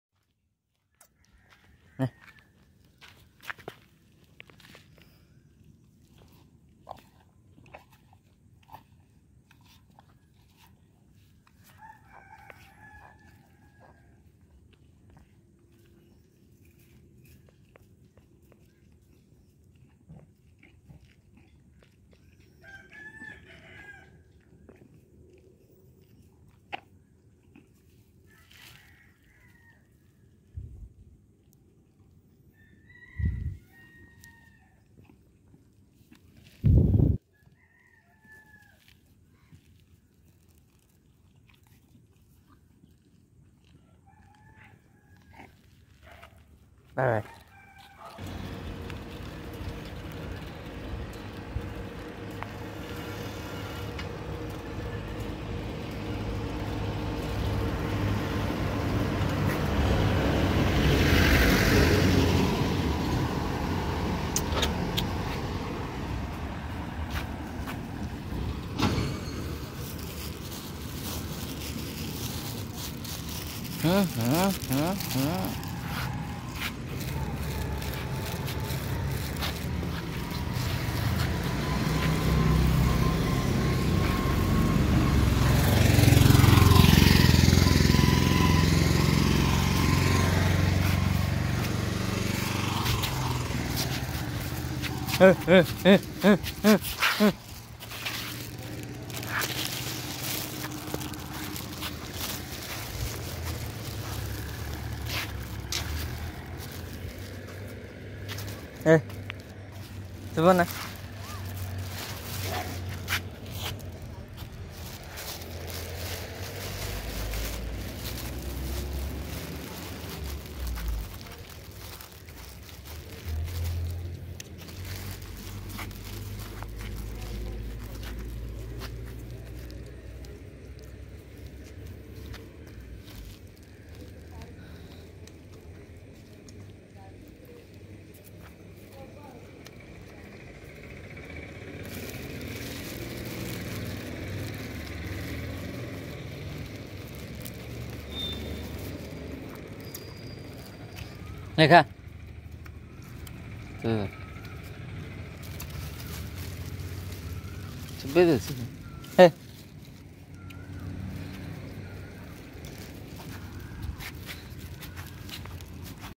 feeding street puppies and dogs | chinku vlogzilla
feeding street dogs in berhampur street lanjipali if u able to help the help them feed them they are innocent animals and they have rights to live